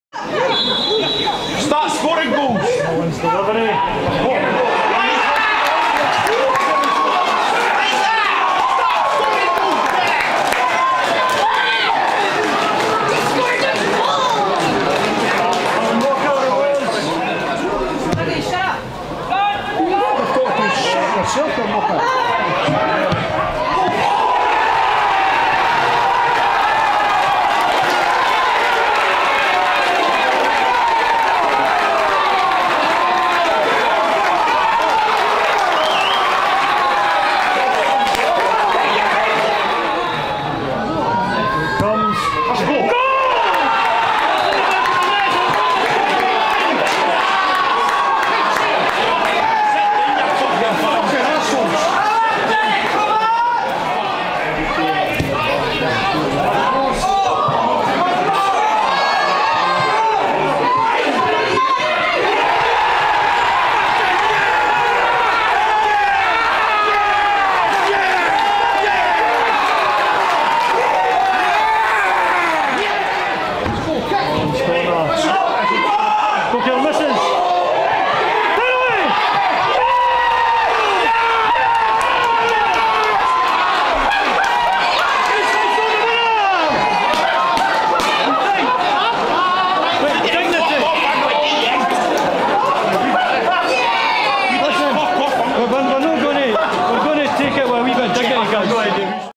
Page seals incredible win for Fifers
Berwick Rangers 2-3 East Fife - League 2 - 15/11/14

East Fife moved into the play-off places after a thrilling match at Shielfield. Allan Walker and Kevin Smith twice had the visitors ahead but Darren Lavery bagged a double, the second in stoppage time, only for Jonathan Page to grab the winner 60 seconds later.